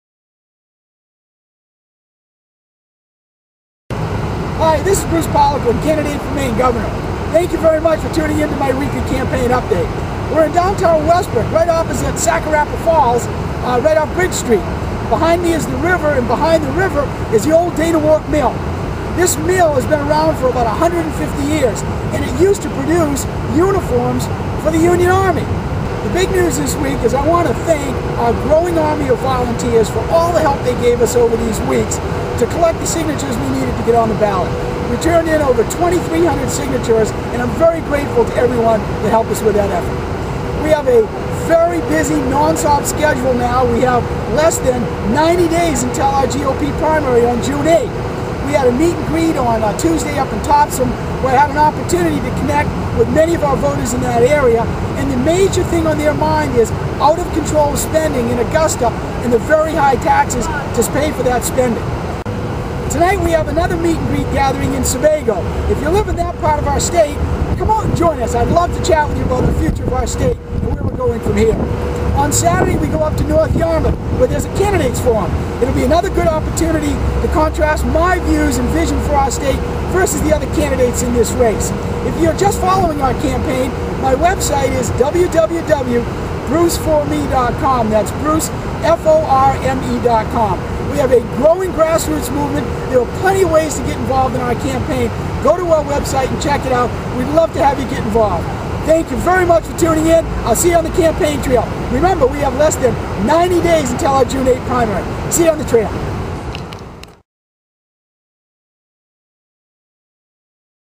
Bruce Poliquin - Weekly Video Update (2010-03-18)
The official weekly video update from Bruce Poliquin, candidate for Maine Governor